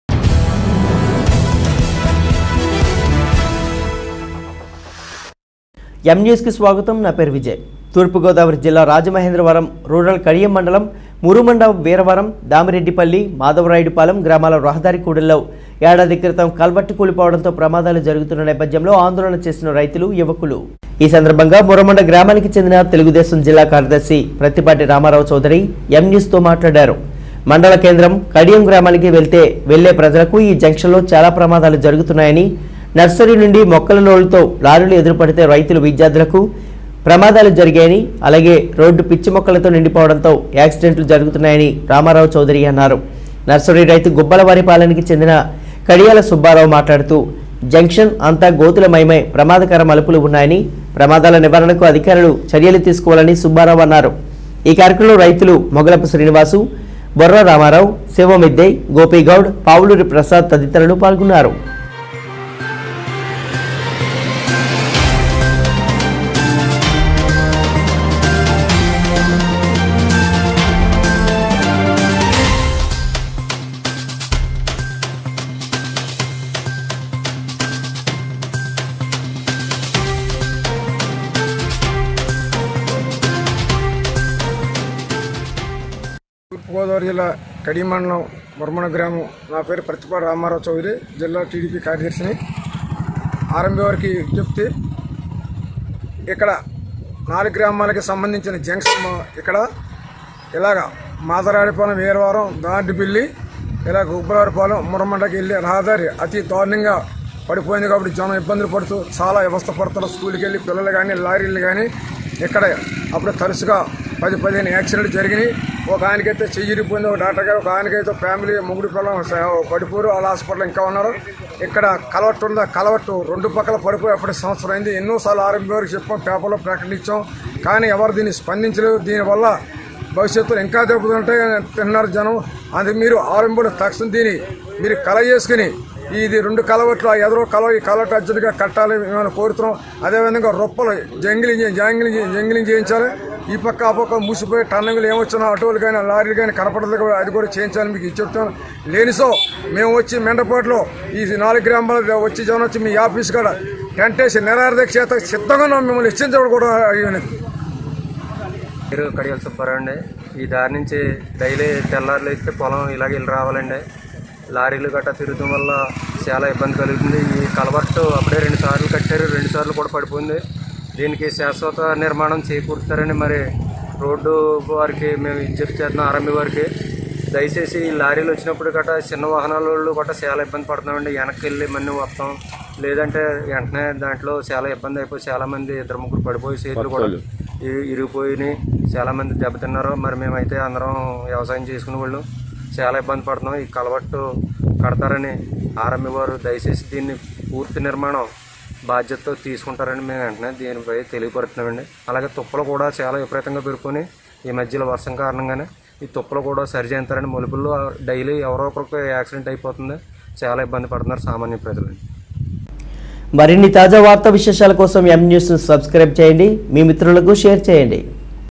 మురమండ, వీరవరం, దామిరెడ్డిపల్లి, మాధవరాయుడు పాలెం, గ్రామాల రహదారి కూడలిలో|| M NEWS TELUGU
తూర్పుగోదావరి జిల్లా రాజమహేంద్రవరం రూరల్ కడియం మండలం మురమండ, వీరవరం, దామిరెడ్డిపల్లి, మాధవరాయుడు పాలెం, గ్రామాల రహదారి కూడలిలో ఏడాది క్రితం కల్వర్ట్ కూలిపోవటంతో ప్రమాదాలు జరుగుతున్న నేపథ్యంలో ఆందోళన చేసిన రైతులు యువకులు... || M NEWS TELUGU